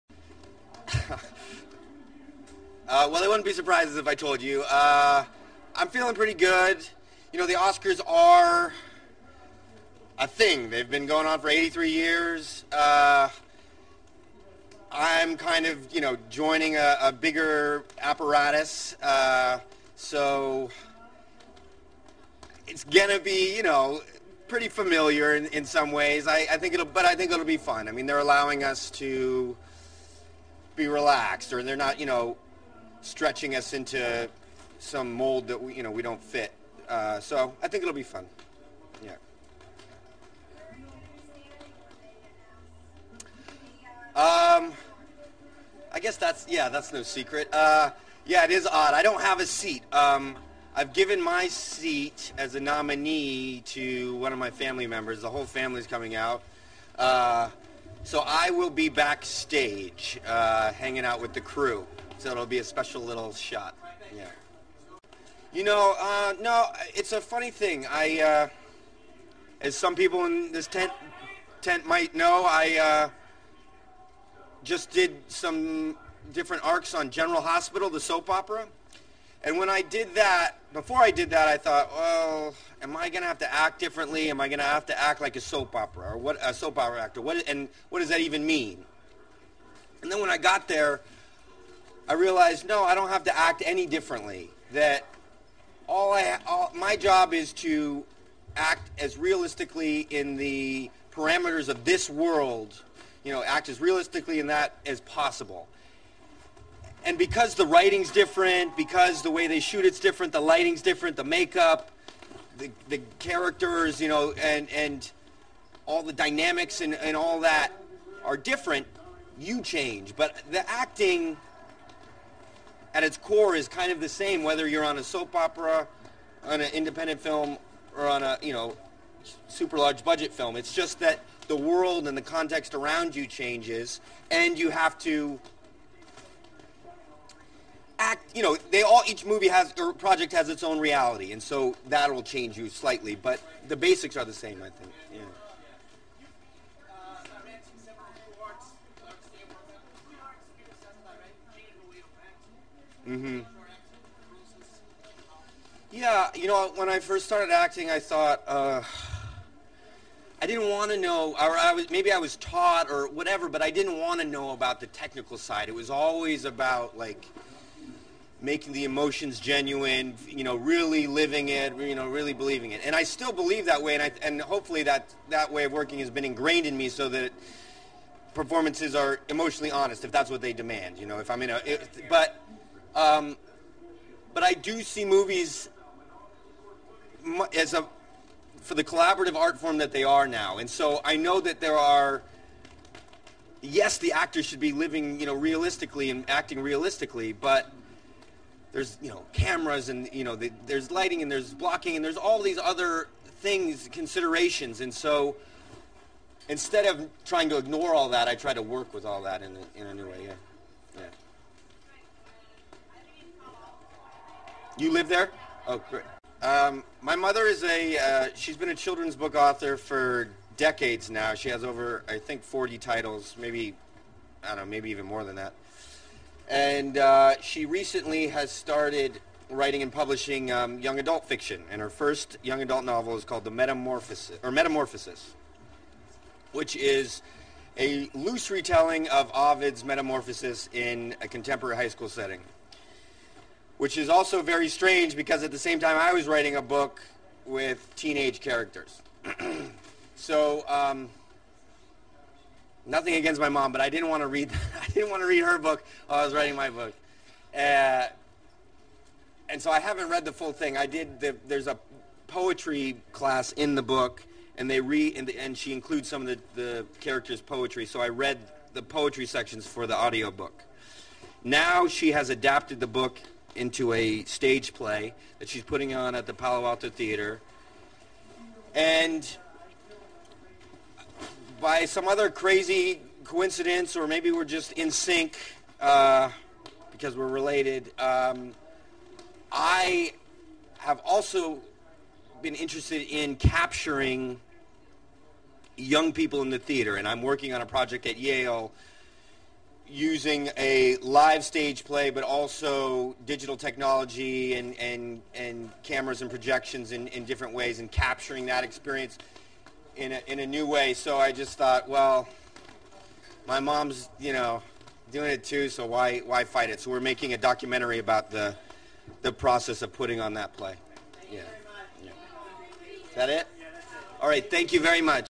James Franco at Film Independent Spirit Awards -Press room /Feb 26, 2011
February 26, 2011- Actor James Franco, winner of the Best Male Lead award for '127 Hours', speaks in the Press room during the 2011 Film Independent Spirit Awards at Santa Monica Beach in Santa Monica, California.
James Franco speaks on expectations with Oscars, his characters, his mother Betsy.